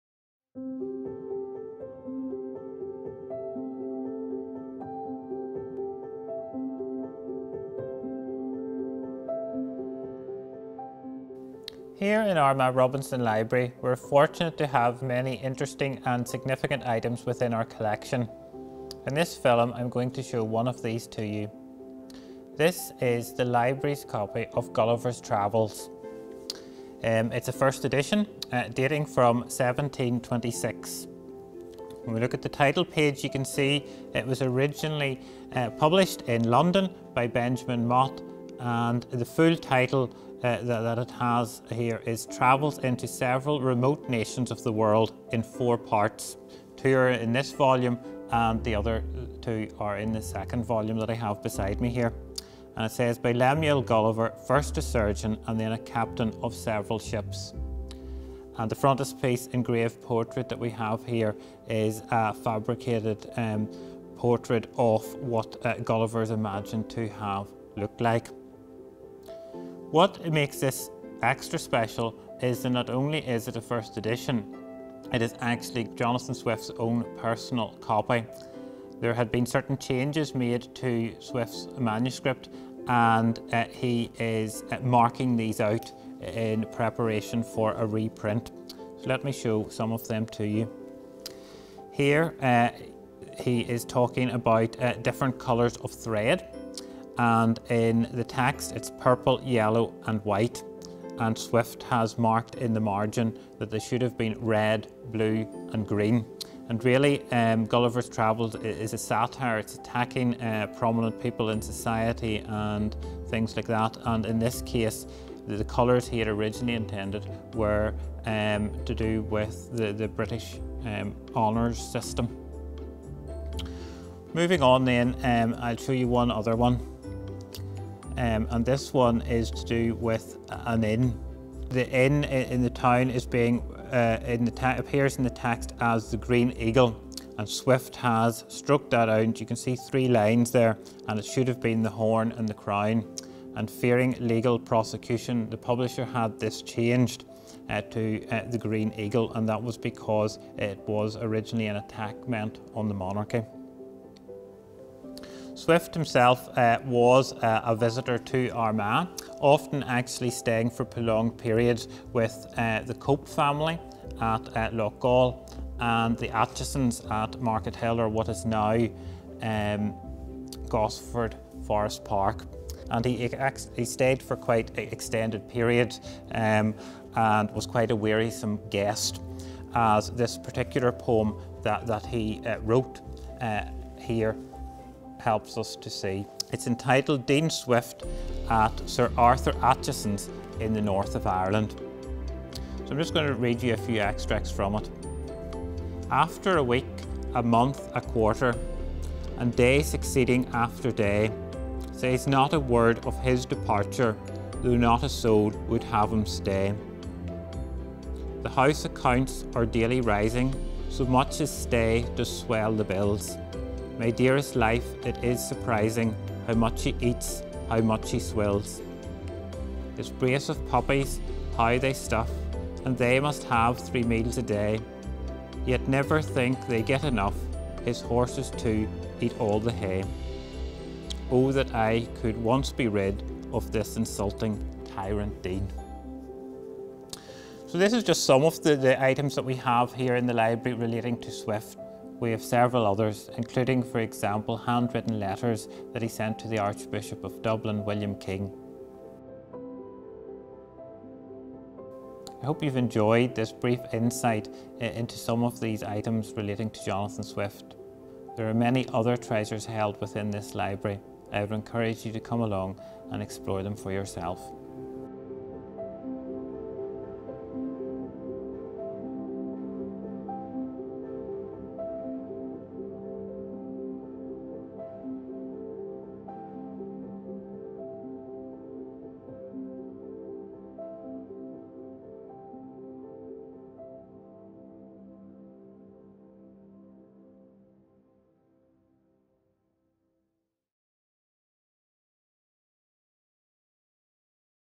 Jonathan Swift Treasures from Armagh Robinson Library
This film, featuring the Director of Armagh Robinson Library, shows Jonathan Swift's personal, annotated copy of 'Gulliver's Travels', one of the treasures of Armagh Robinson Library. It then discusses Swift's visits to Armagh before concluding by showing various manuscript and printed items that the Library has relating to the famed author.